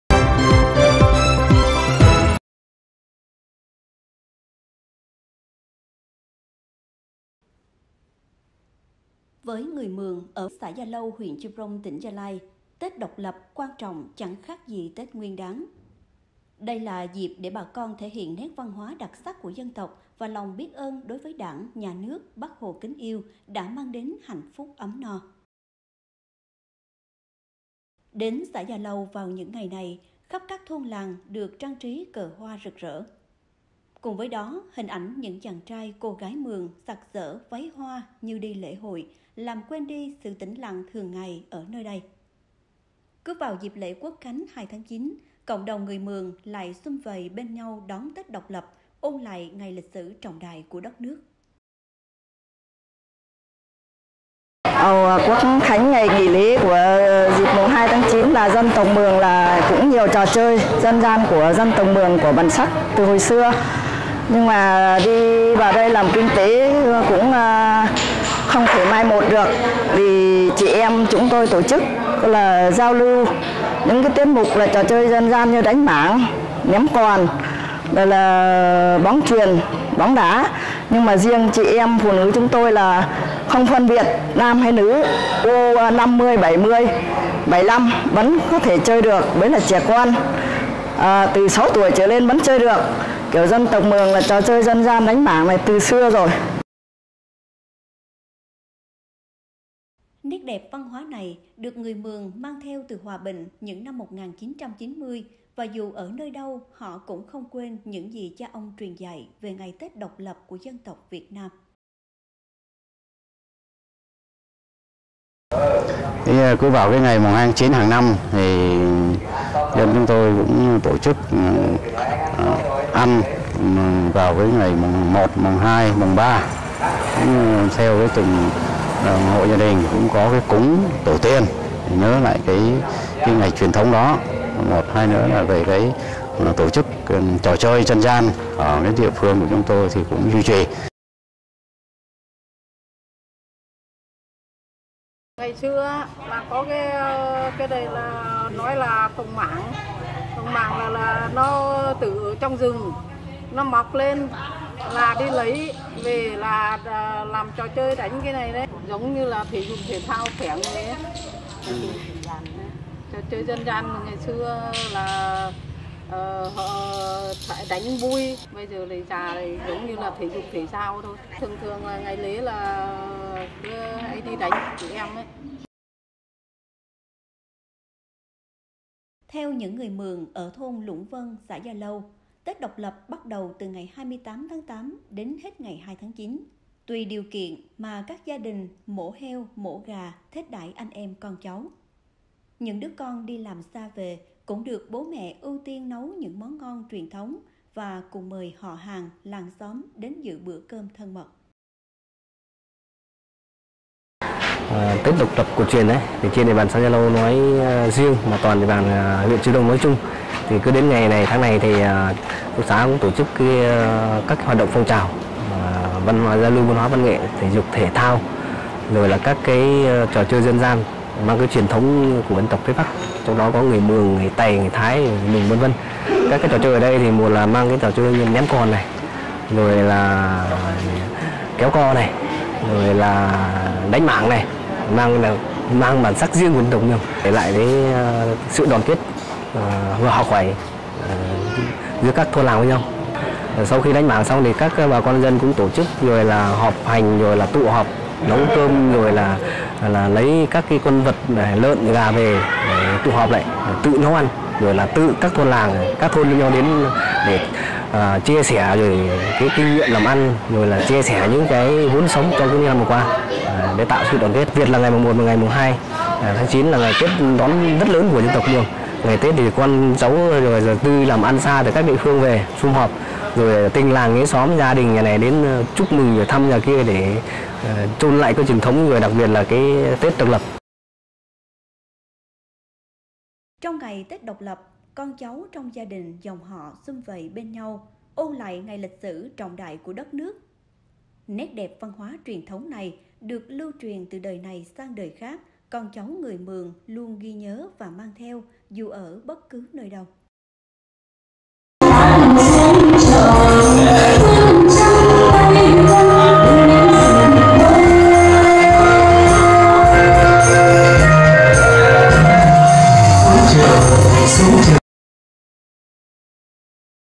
Người Mường ở Ia Lâu vui Tết Độc lập
Với người Mường ở xã Ia Lâu (huyện Chư Prông, tỉnh Gia Lai), Tết Độc lập quan trọng chẳng khác gì Tết Nguyên đán. Đây là dịp để bà con thể hiện nét văn hóa đặc sắc của dân tộc và lòng biết ơn đối với Đảng, Nhà nước, Bác Hồ kính yêu đã mang đến hạnh phúc, ấm no.